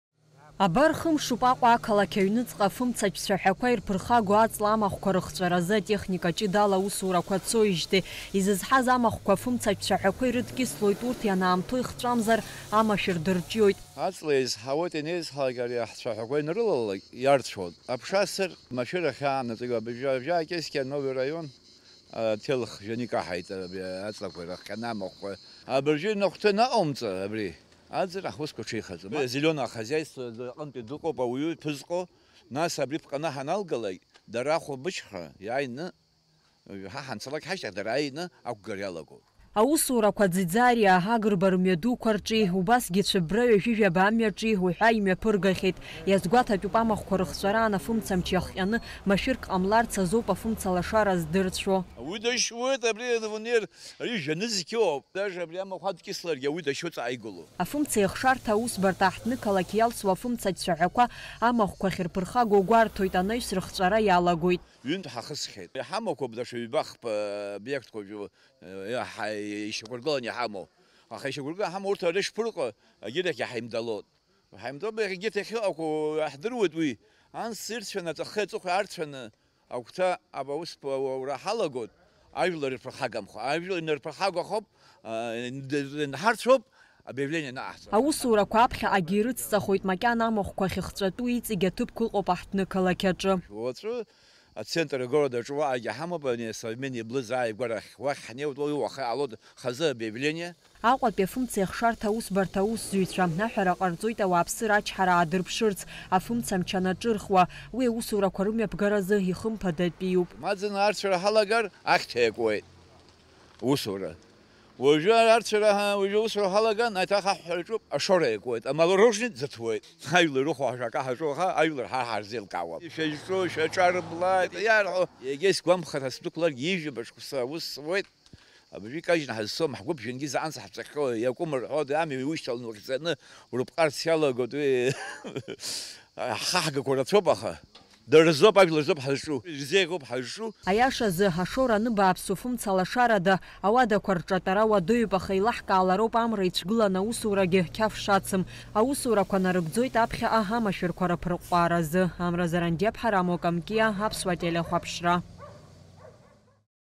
Арҭ амшқәа рзы аамҭа-аамҭала аҳҭнықалақь ахәҭақәа рҿы афымцалашара дырцәоит
Уи  зыхҟьо, Аҟәатәи афымцеихшарҭа аусбарҭа афымцатә цәаҳәақәа ирҧырхагоу аҵла амахәқәа рыхҵәара азы аусурақәа ахьымҩаҧырго ауп. Иҳәатәуп, арҭ аусқәа  рынагӡаразы Аҟәатәи  ариаҵәаратә нхамҩа  аусбарҭа  иазоужьын  шә-нызқь мааҭ аҧара. Ажәабжь иацырҵоит ҳкорреспондентцәа.